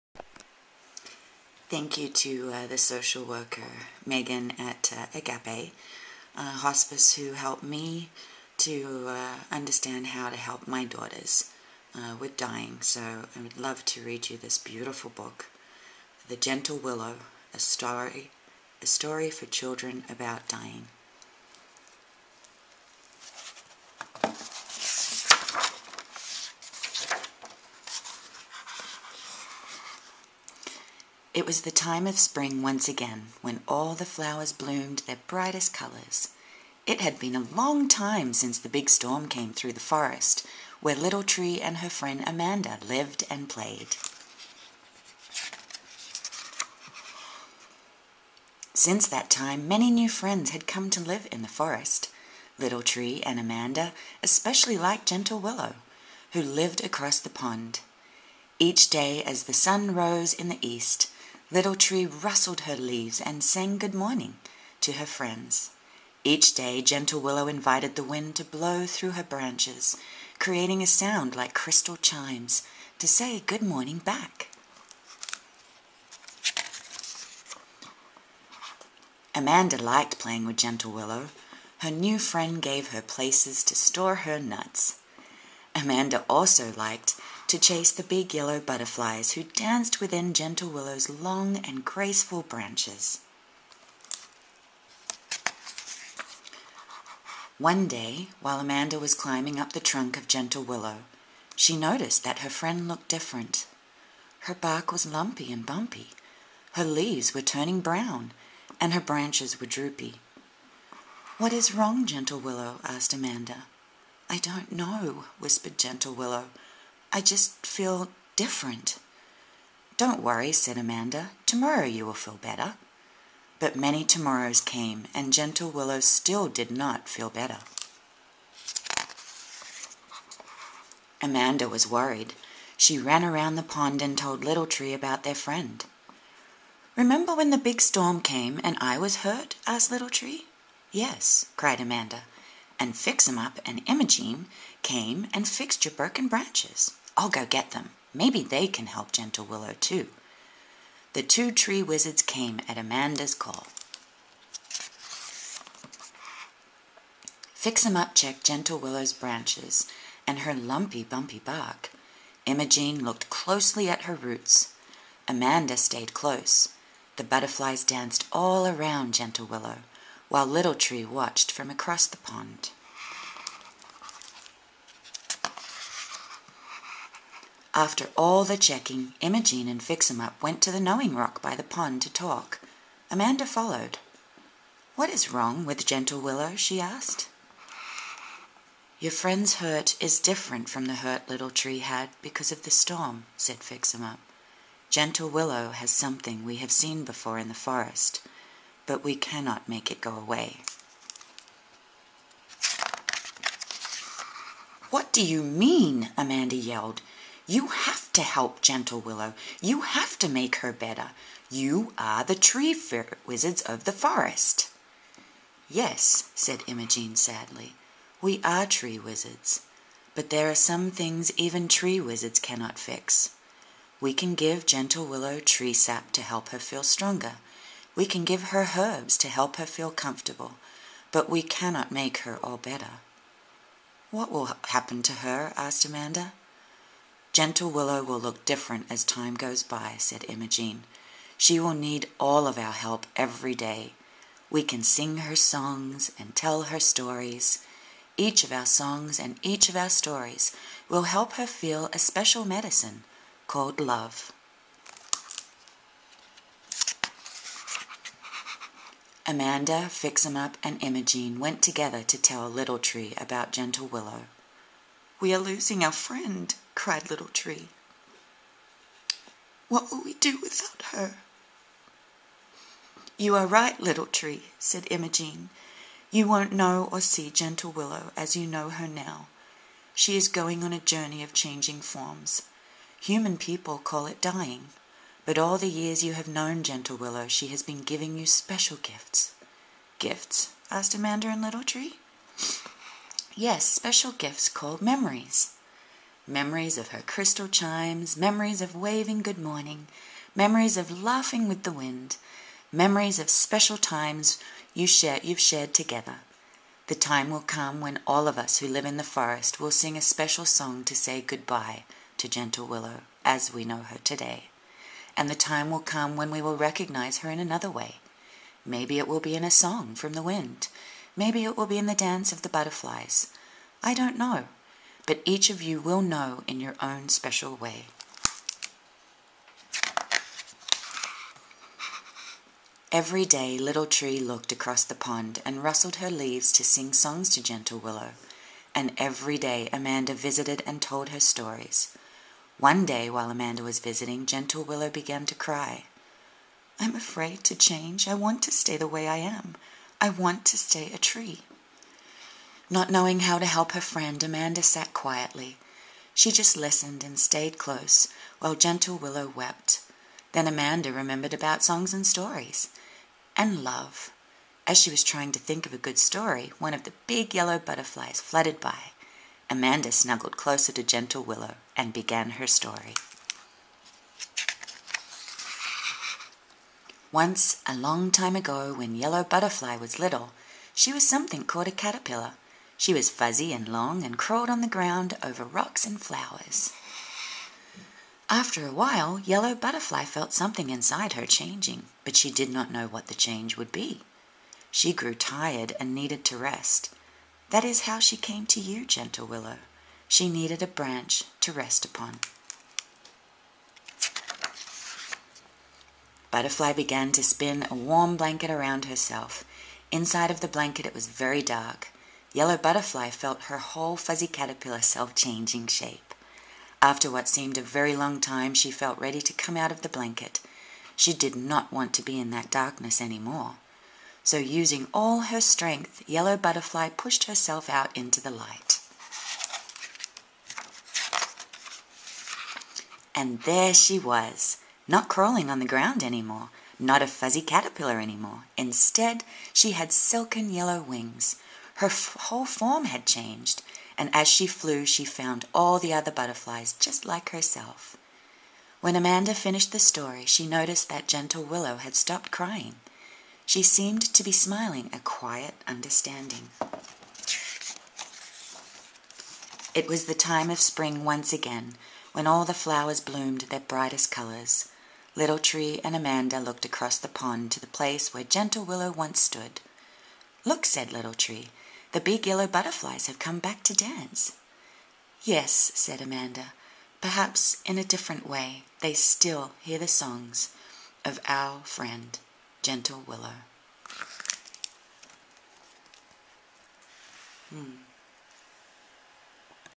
Gentle Willow
A Beautiful story for children about dying...
I also believe a beautiful story for ALL ages xox
You can purchase this lovely book on Amazon